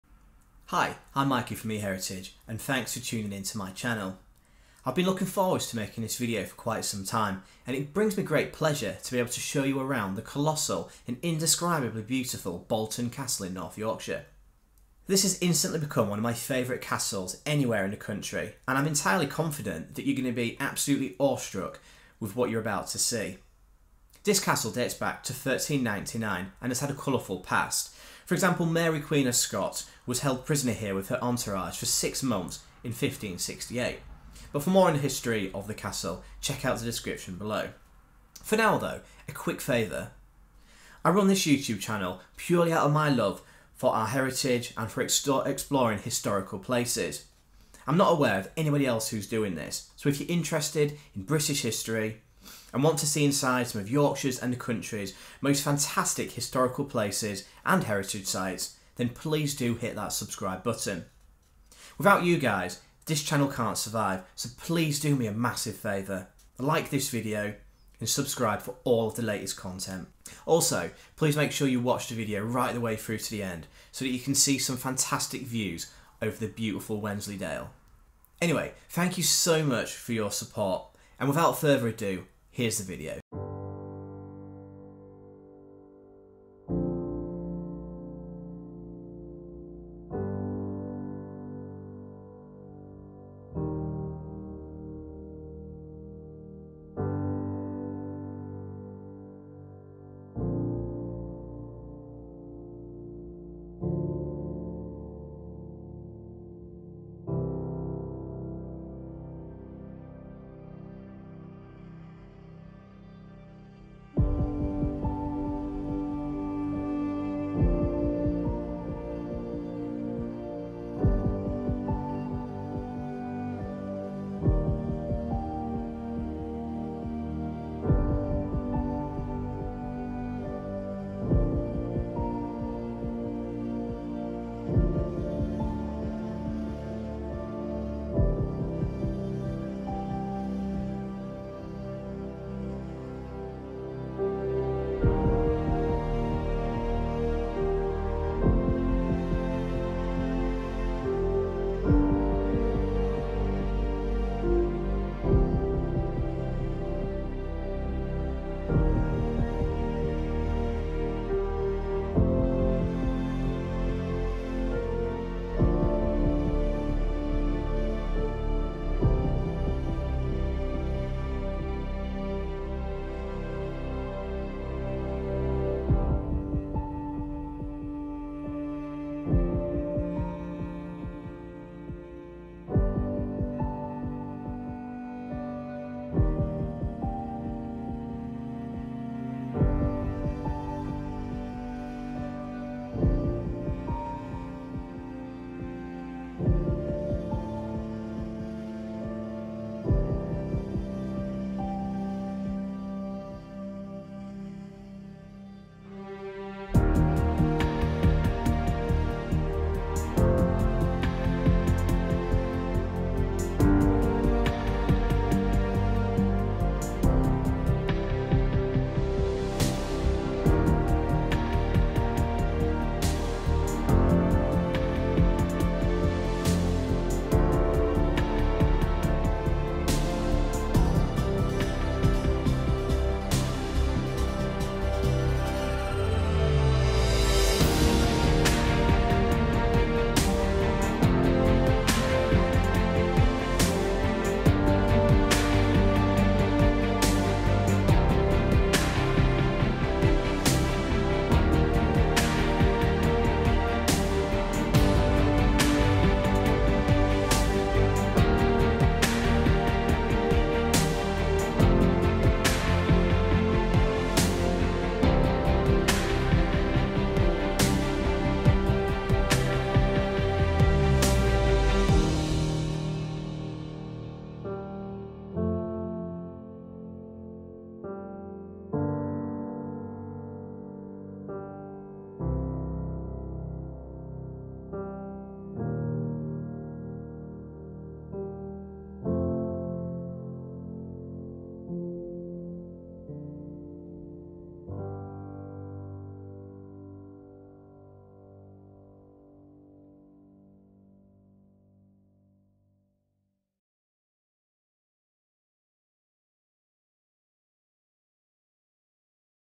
Inside Medieval Bolton Castle | 4K Castle Tour
Bolton Castle is one of the best Castles to tour in England, and the UK. In fact, Bolton Castle is one of the finest Medieval Castles still in existence in the world! Bolton Castle also represents one of the finest family days out in Yorkshire. After a visit with my family, and armed with my Fujifilm X-T4 camera, I put together this short Cinematic Castle Tour. If you want to see inside Bolton Castle, then this video is for you!

history of Bolton Castle -

Set within stunning surroundings and built on the site of an earlier Manor House, Bolton Castle was built for Sir Richard, the first Lord Scrope. Constructed between 1379 and 1399, Bolton Castle has seen some action over its lifetime. Of significance is that Mary Queen of Scott’s was imprisoned here for six months in 1568. The castle was actually set on fire by Henry VIII in the sixteenth century and was marginally slighted during the civil war

Bolton Castle is slated as being one of the best-preserved medieval castles in England, and I would be inclined to agree. Whilst much of it is in ruin, the owners have seemingly invested a considerable amount of time and money in bringing parts of the castle back to life. In fact, there are plenty of indoor areas to explore, such as the Great Hall, Lord Scrope’s bedroom, and the room that Mary Queen of Scots and her entourage were held in.

I have a particular interest in Bolton Castle as it was allegedly designed by the same architect as Sheriff Hutton Castle. I live very close to Sheriff Hutton and have been fascinated by it for years. It’s hard to imagine what Sheriff Hutton must have looked like based on its current state, however if we look to Bolton Castle then we get a good impression of how magnificent and imposing it must have been!

Bolton Castle is my favourite castle in Yorkshire and is far larger than you expect. Try to go on a good day if you can so that you can enjoy the fantastic views from the battlements.